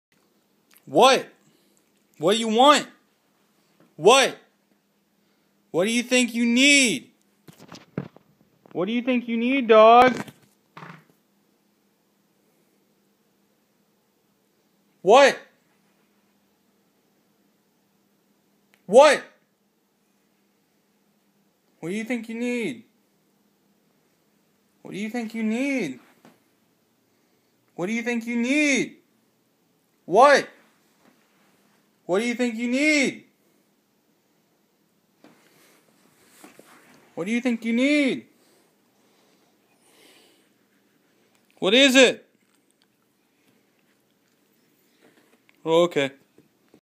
Dog believes he needs something
My dog thinks he needs something but doesn't give any detail as to what it was. I had to do some interrogating, and this was the result.